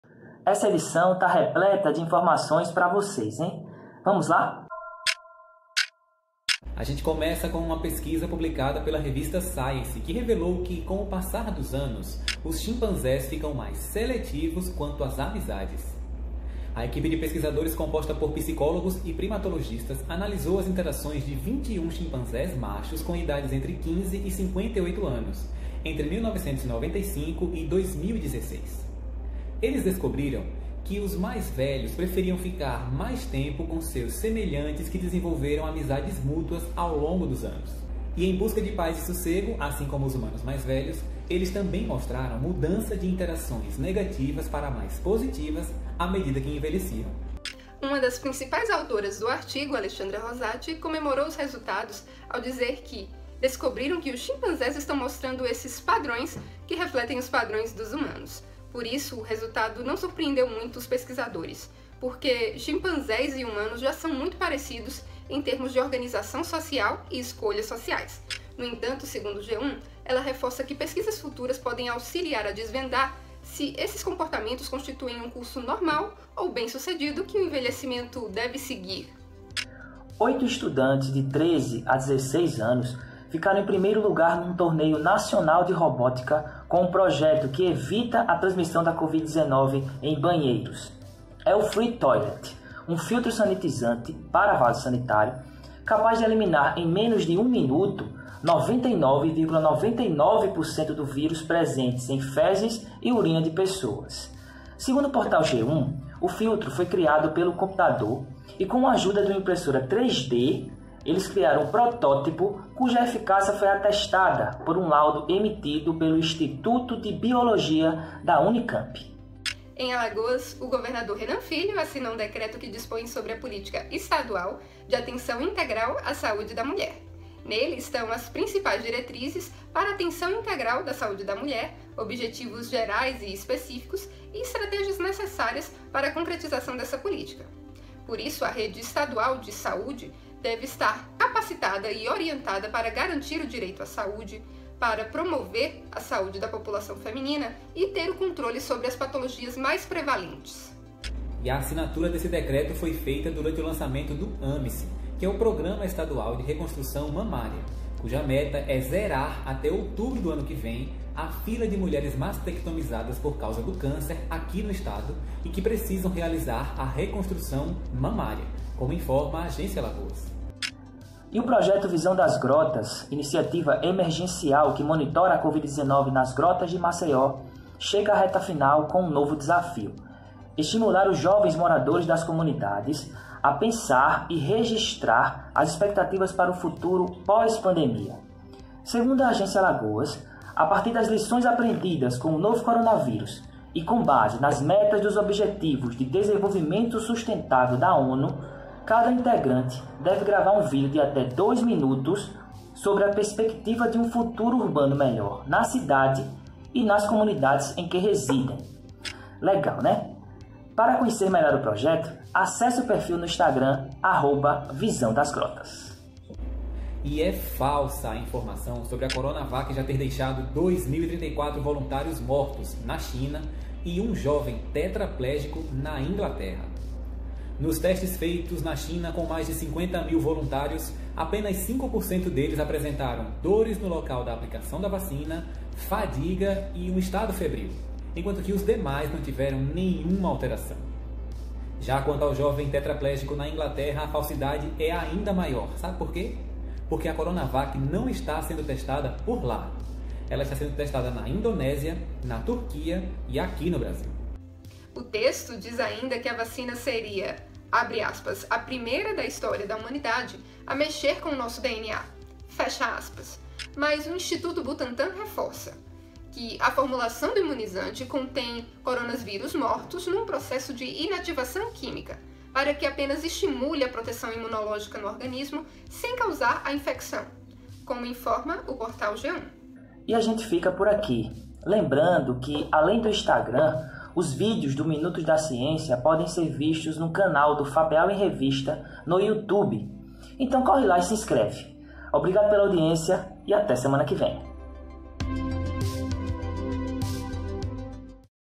Minutos da Ciência #22 - Confira as notícias desta edição (30/10/2020)
🦧 Será que você é um pouco... Chimpanzé? Calma, dá o play que a gente explica com uma pesquisa da RevistaScience.

🗣 Também falamos sobre o Freetoilet, um projeto de estudantes de 13 a 16 anos capaz  de eliminar o vírus da Covid_19 em banheiros e que foi premiado num Torneio Nacional de Robótica.

🔛 E mais: A Política Estadual de Atenção à Saúde da Mulher em Alagoas e mais uma FakeNews sobre o potencial da Coronavac.